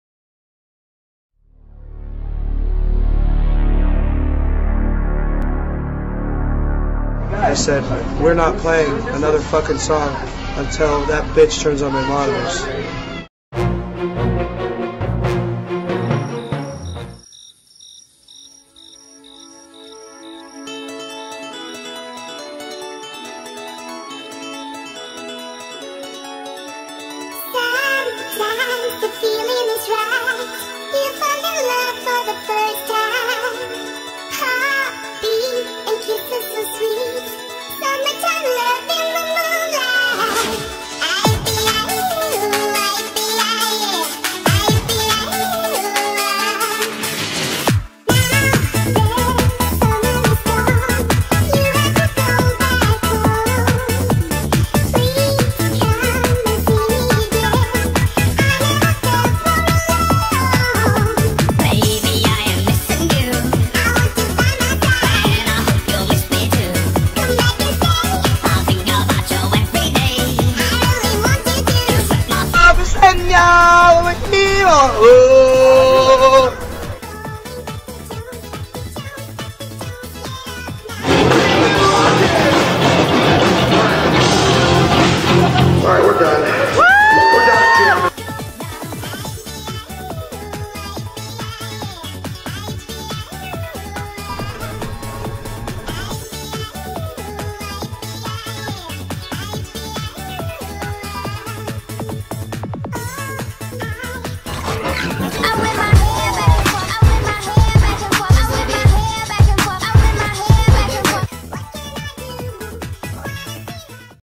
Ronnie Radke's Dare...
Ronnie is now out of jail but wanted to get payback...